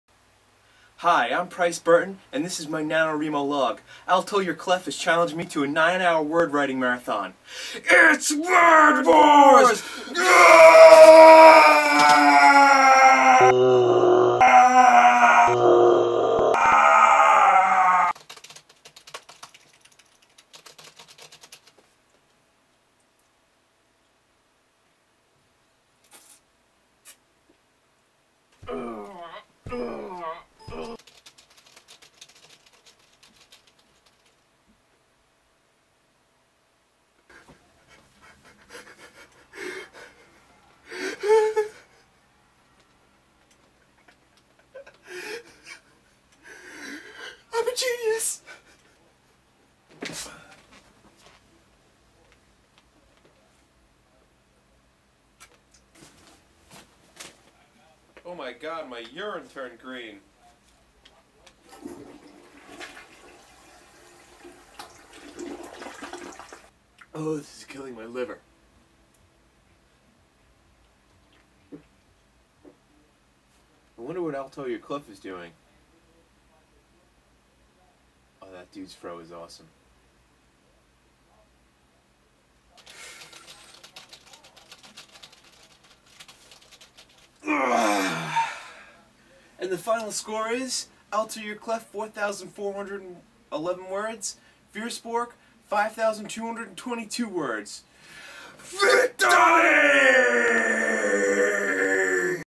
Nanowrimo18 -epic word battle-
hey nano wrimos the epic show down between me and altoyourclef